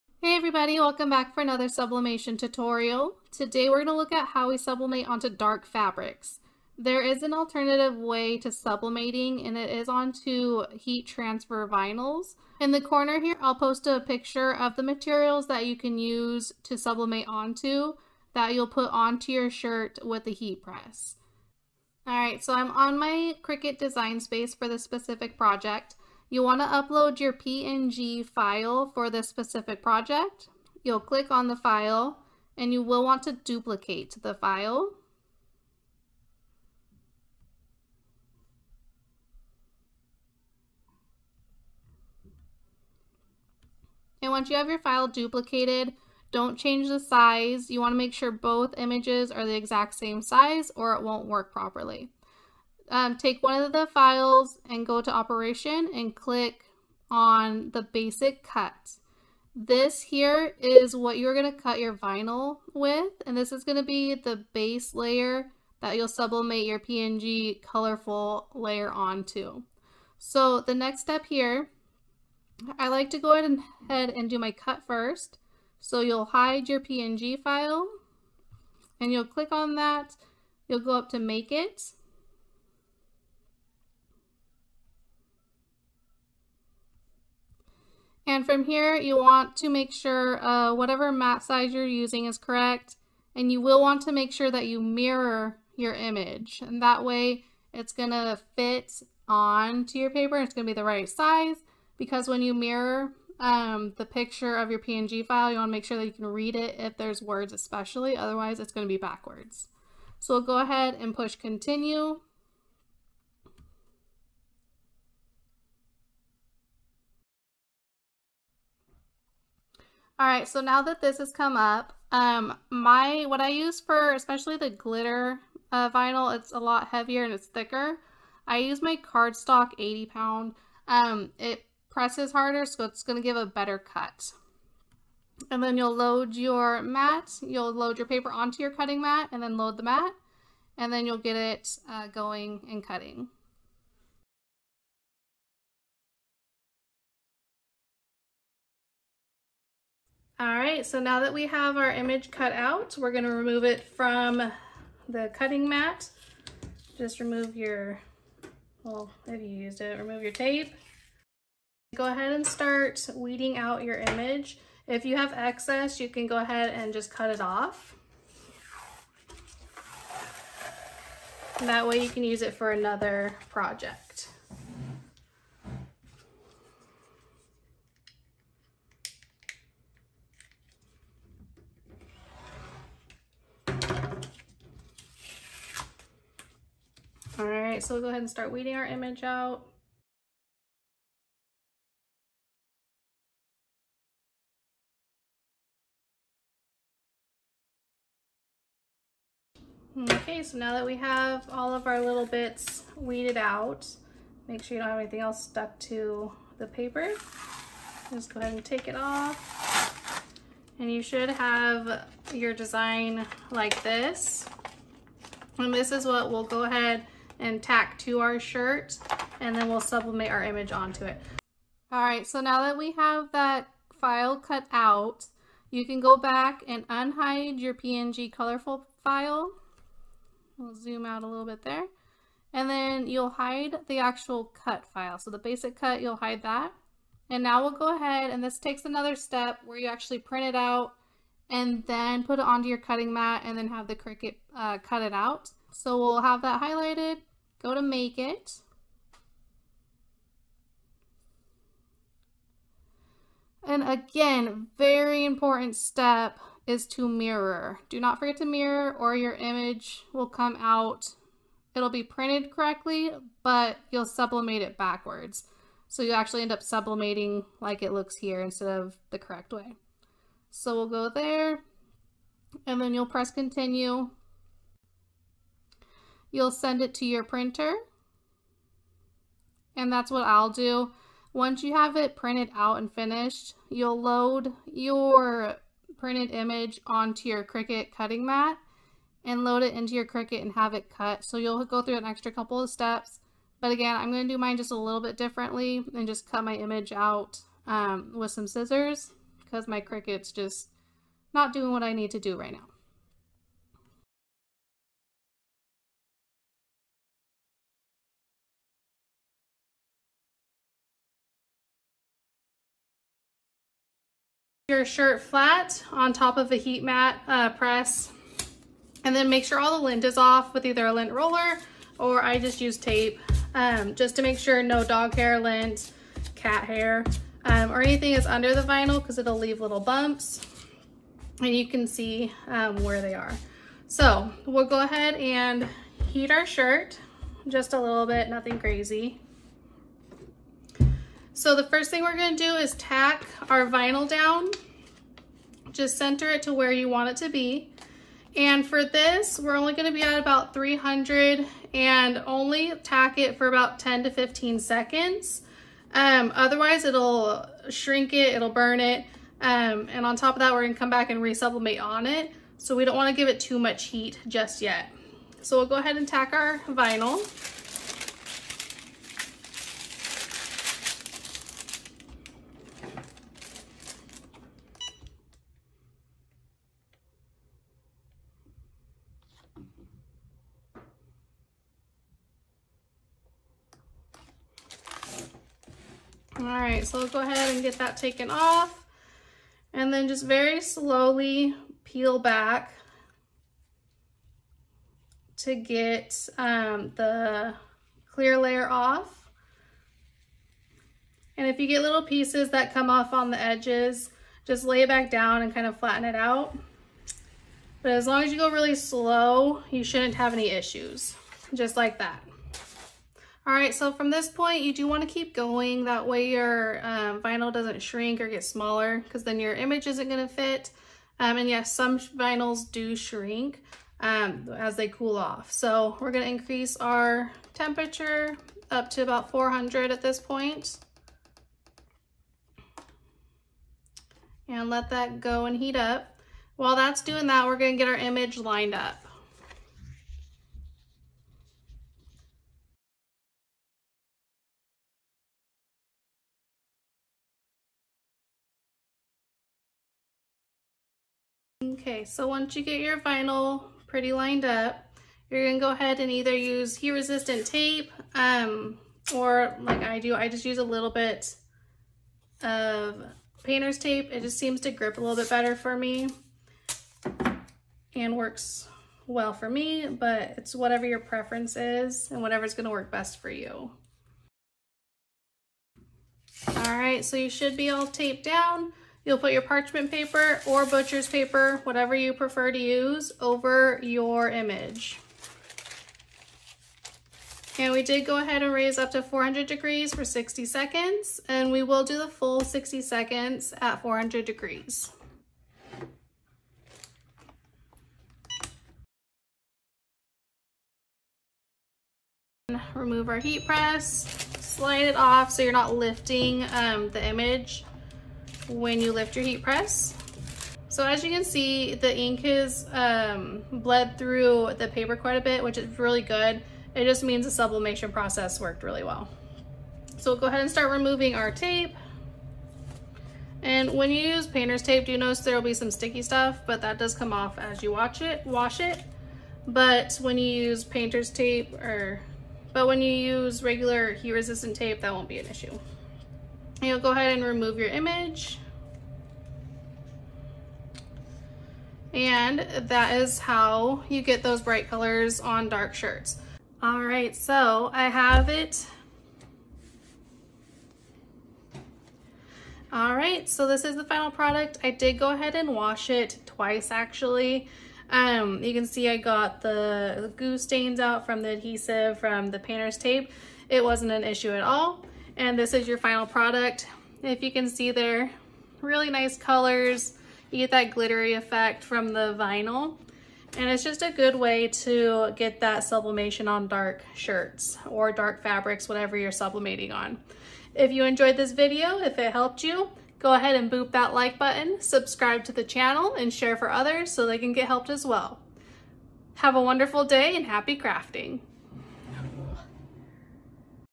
How to Sublimate on Dark Shirts
Who says you can make a DARK sublimated SHIRT ! This technique is so EASY and FUN, allowing you mor freedom with the colors of fabrics that you can sublimate onto! I will review the exact process that I have found to work the best! I have tested techniques so you won't have to!

My favorite place to get 1,000,000's of designs!!

* ANY type of t-shirt will work for this project. I used a 100% cotton shirt from Walmart.

* Parchment paper: (I use the Walmart brand. You can use what works best for you)

* Cricut Iron - On Glitter Vinyl - I get my from Walmart, but most craft store sell it as well.

*Cricut Explore Air 2 machine

* Cricut heat press 8 x 8 and Cricut heat pad

* Cricut Design Space - this method can be used with other cutting machines as well.

* Sublimation printer  - Epson ET 2803

* Sublimation ink - Hippoo Ink -

* Sublimation paper - Sawgrass Sublimation Paper (Yes, it works wonderfully in the Epson ET 2803 printer)

for more video tutorials! Happy crafting!